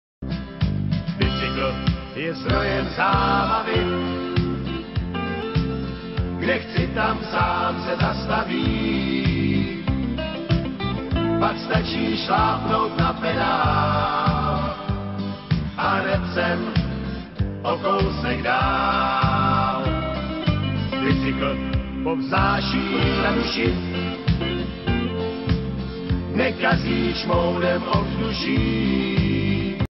Bicykl a cyklostezka
proč by užil cyklostezku, když se může mrcasit v provozu.
HUDBA: PLAVCI